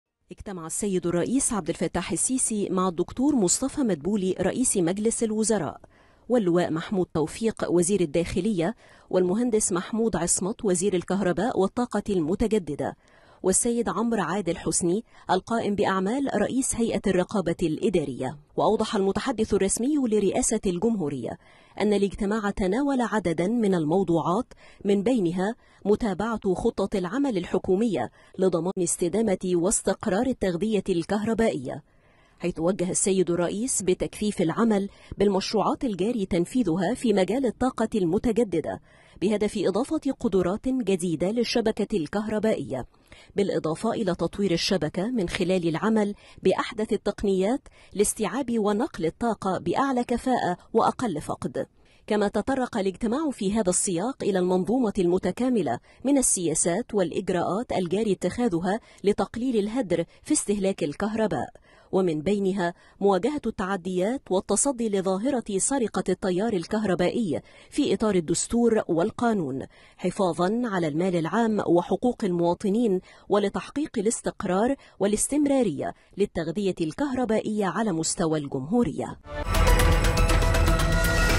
الرئيس السيسي وجه بتكثيف العمل بالمشروعات الجاري تنفيذها في مجال الطاقة المتجددة
الرئيس السيسي وجه بتكثيف العمل بالمشروعات الجاري تنفيذها في مجال الطاقة المتجددة بهدف إضافة قدرات جديدة للشبكة الكهربائية